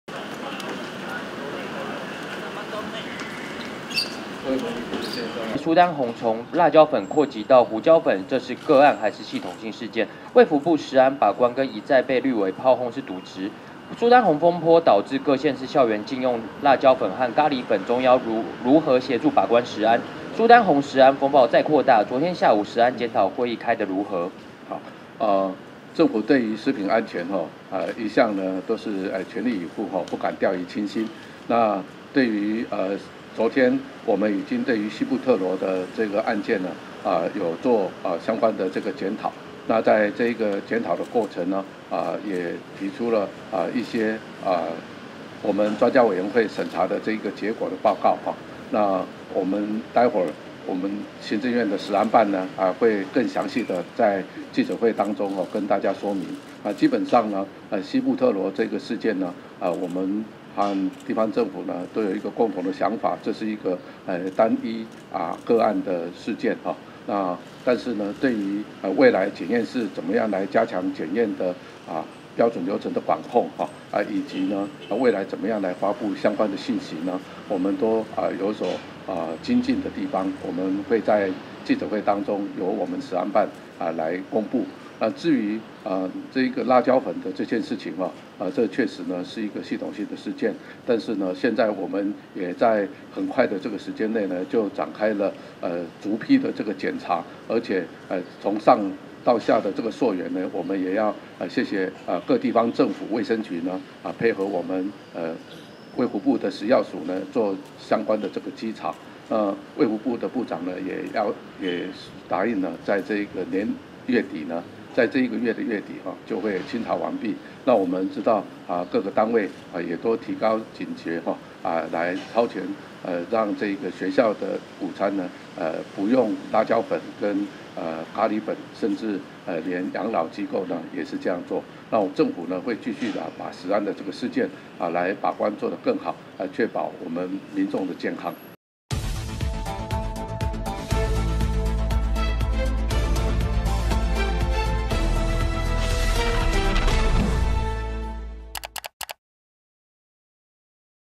蘇丹紅風暴擴大！陳建仁認「系統性事件」：月底前清查完畢
記者劉秀敏／台北報導

蘇丹紅辣椒粉風暴擴大，全台22縣市陸續宣布營養午餐暫停使用辣椒粉、咖哩粉，行政院長陳建仁今（8）日受訪坦言「這確實是一個系統性的事件」，政府也在很快時間內展開逐批檢驗、從上到下溯源，本月底就會清查完畢。